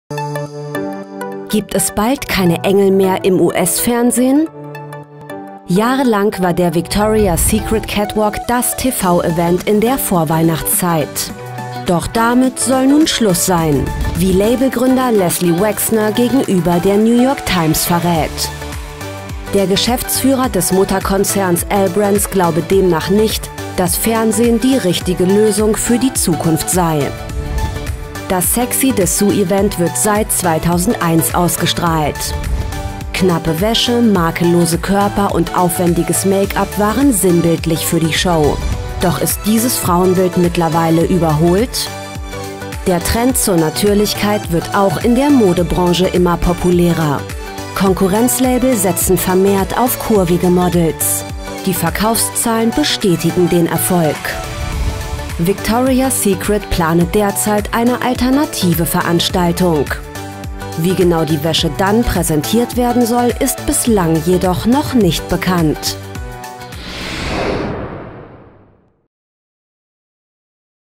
„Victoria’s Secret“-Show wird aus TV verbannt
Jahrelang war der „Victoria’s Secret“-Catwalk das TV-Event in der Vorweihnachtszeit. Doch damit soll nun Schluss sein, wie Labelgründer Leslie Werner gegenüber der New York Times verrät.